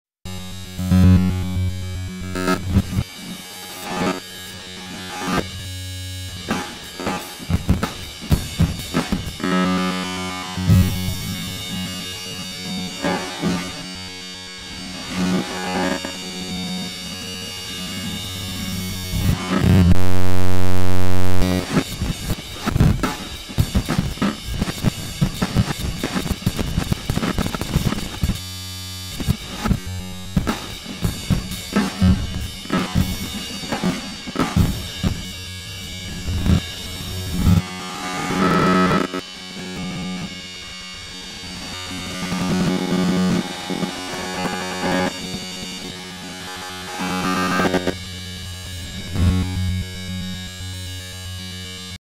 RxMarkers - Early Test
First working version of the Lua script for moving slice markers.